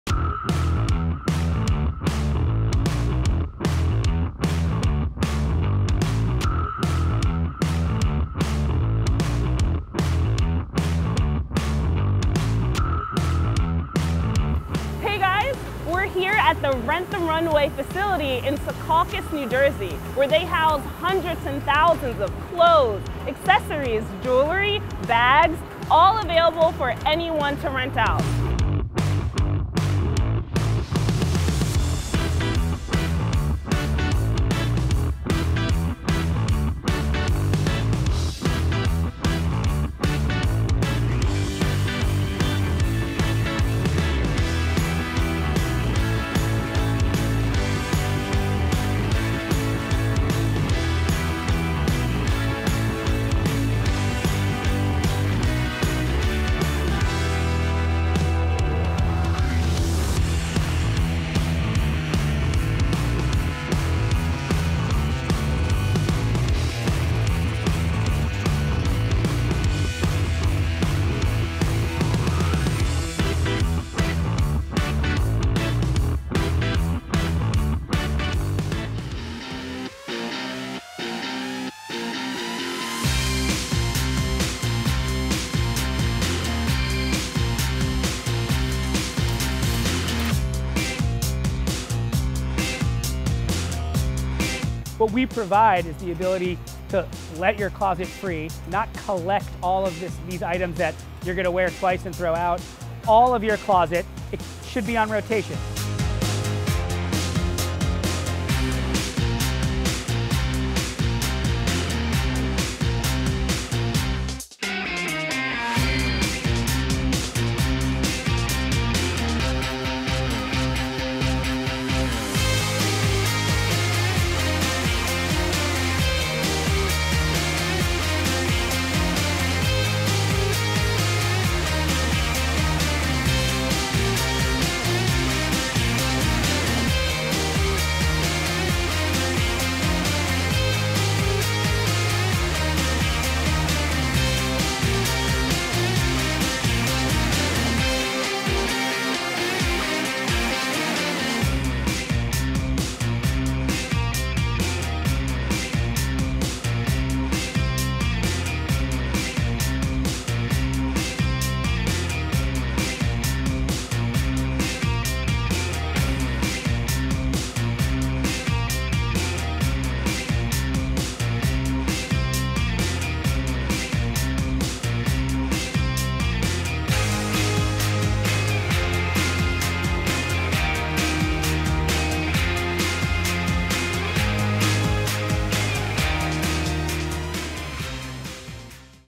Inside The Rent The Runway Warehouse
Rent the Runway is like a library for rentable designer clothes and accessories. The company's warehouse holds 450,000 items customers can check out online, on the app, or in-store. You cant rent in three ways: checking out an item, choosing four items for $90 a month, or gain access to an unlimited wardrobe for $160 a month.